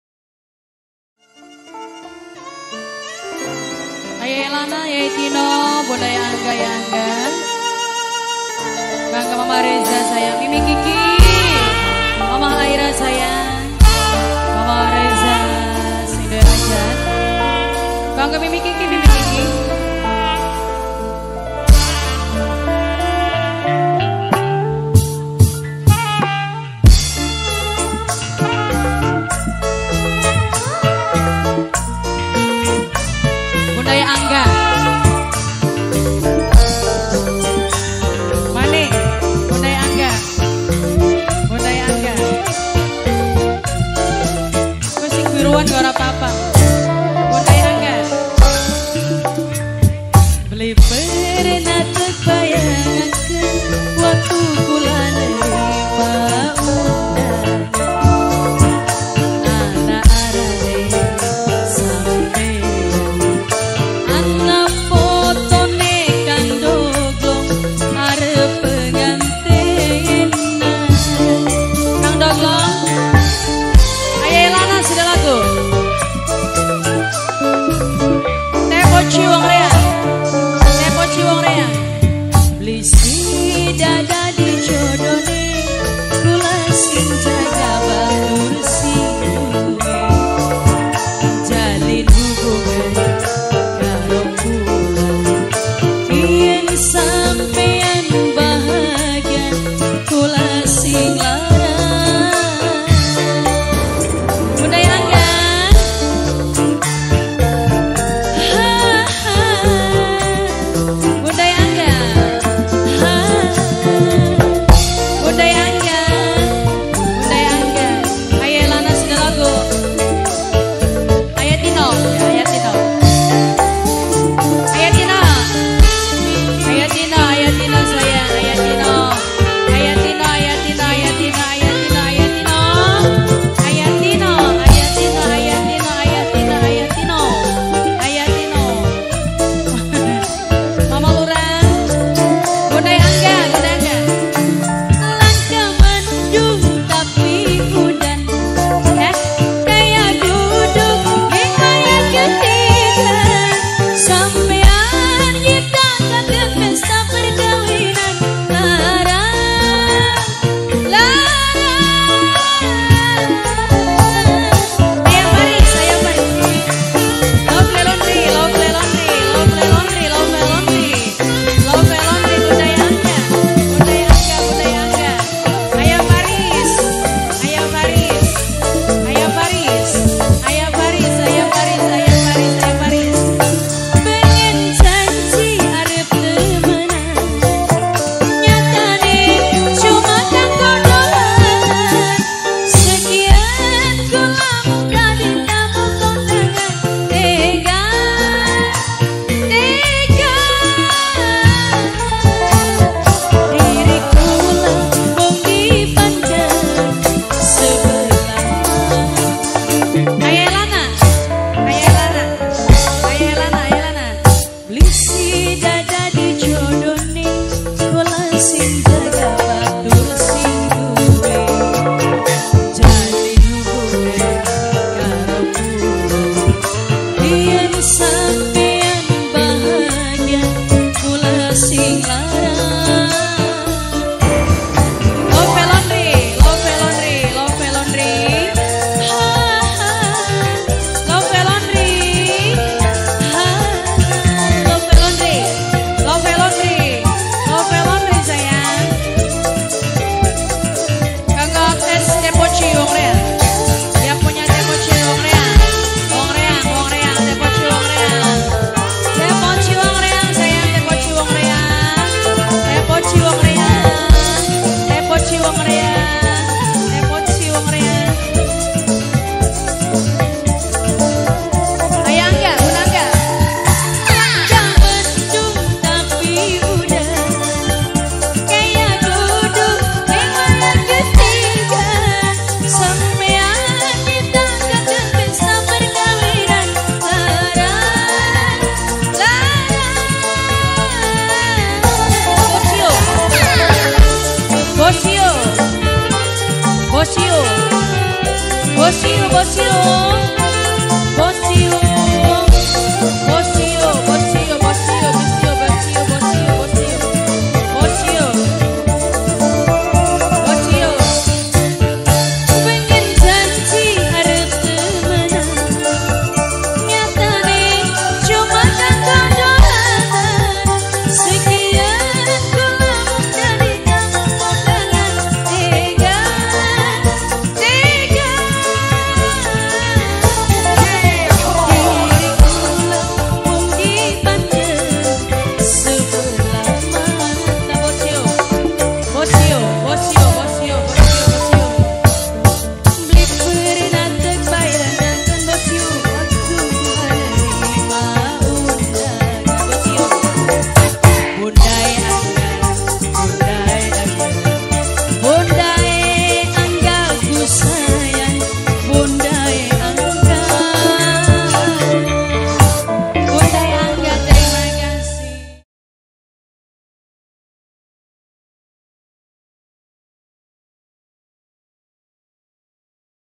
TAMU KONDANGAN # PSM PUTRA SURTI MUDA# LIVE SHOW LOBENER
DOKUMENTASI HAJATNYA KELUARGA BESAR BAPAK JUNEDI (JUNED) / IBU SURENI (SUR) KHITANAN MUHAMMAD REZA
HARI KAMIS 10 NOVEMBER 2021
JL.PANCORAN MAS DESA LOBENER BLOK BUYUT KARANG SUWUNG RT.02 RW.03 KEC. JATIBARANG - INDRAMAYU
DIMERIAHKAN OLEH ARAK ARAKAN MANUK DANGDUT "PSM" PUTRA SURTI MUDA DARI DESA BONGAS PENANGGUL KEC. BONGAS - INDRAMAYU

DOKUMENTASI DIABADIKAN DAN KERJA BARENG RA STUDIO PRO "MEDIA SHOOT & LIVE STREAMING"
DARI DESA LOBENER Gg.PUTRI KEC. JATIBARANG - INDRAMAYU CONTAC PERSON SMS/WA : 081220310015